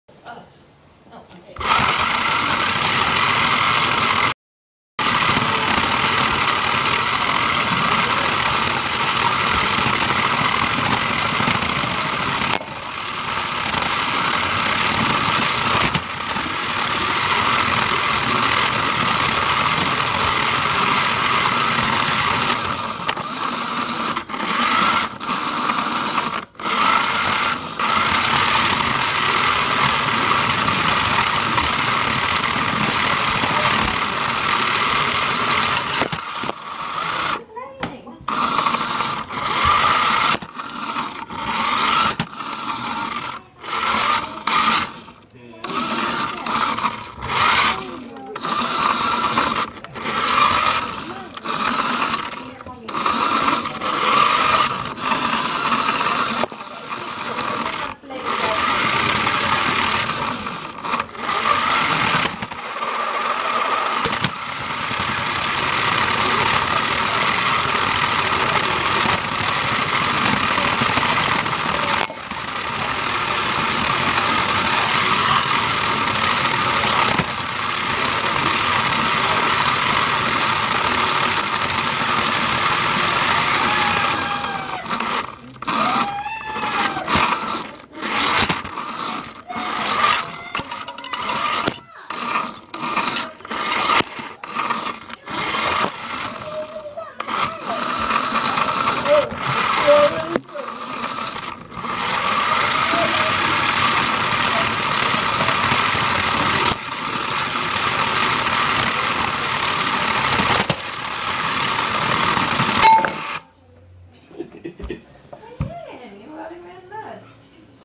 iPod on a remote control car
I tried to drive it via FaceTime on my iPhone but the lag made it too tricky. Lots of fun though